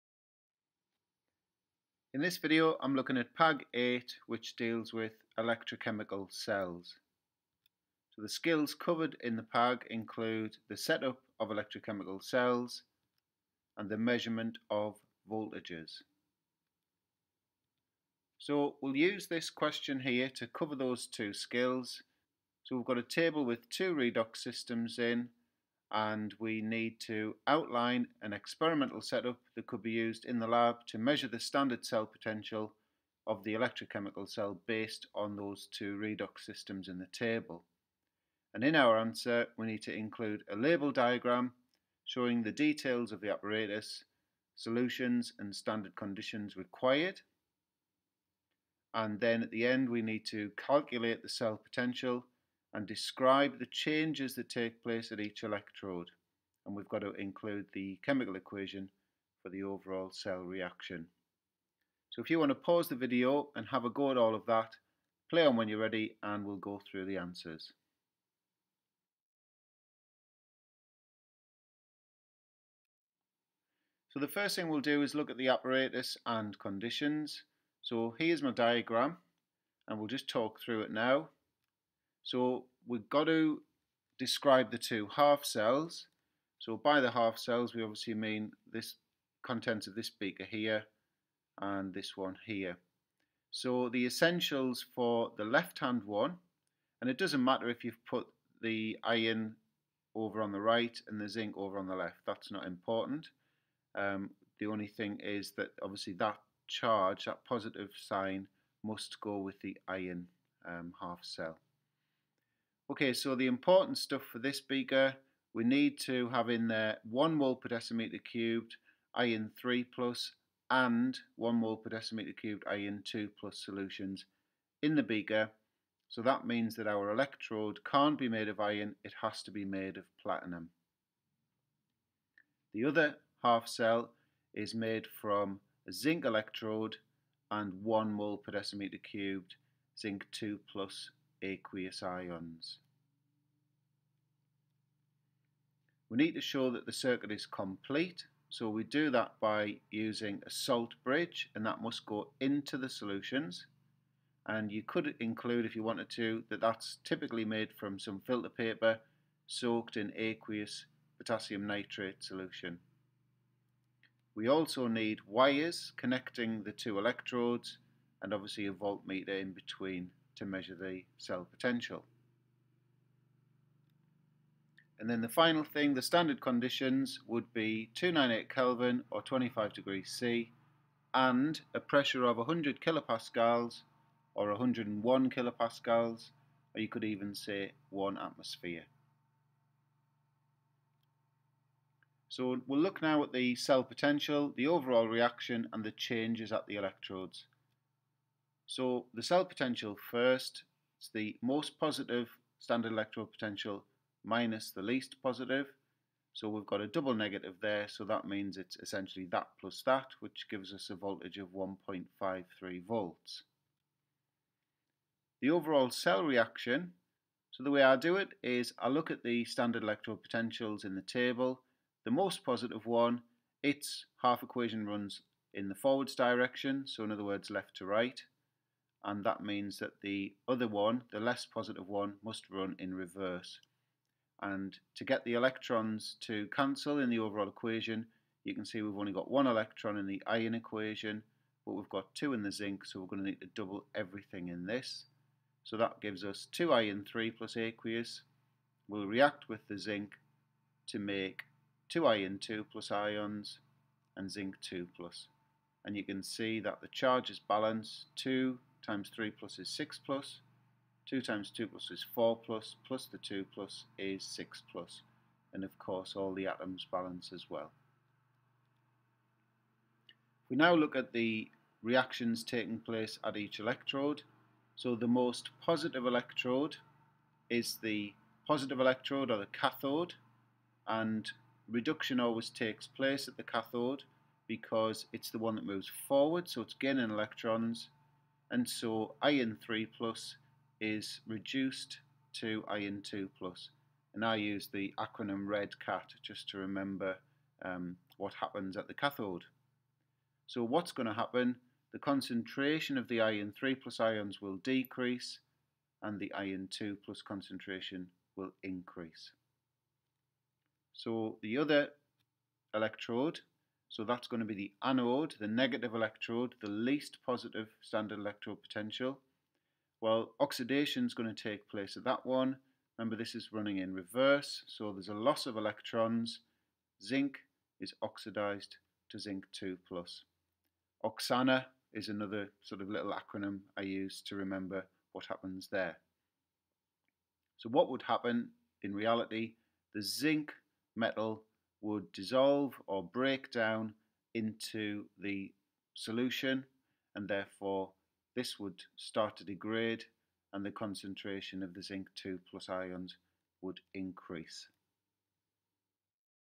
PRACTICAL SKILLS - PAG 8 - ELECTROCHEMICAL CELLS
Video uses an exam style question to look at the experimental set up for the construction of an electrochemical cell followed by calculation of the cell potential and a look at the changes taking place at each electrode.